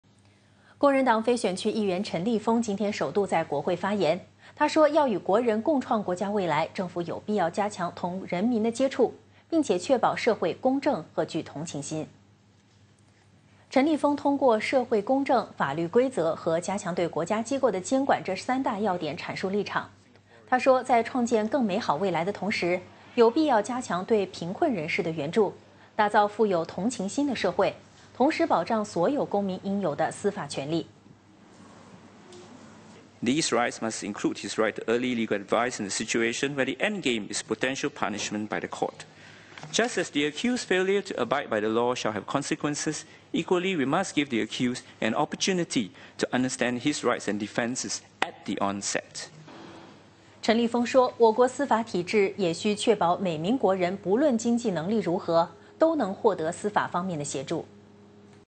陈立峰：创建美好未来 须确保社会公正
工人党非选区议员陈立峰，今天首度在国会发言。他说，要与国人共创国家未来，政府有必要加强同人民的接触，并确保社会公正和具同情心。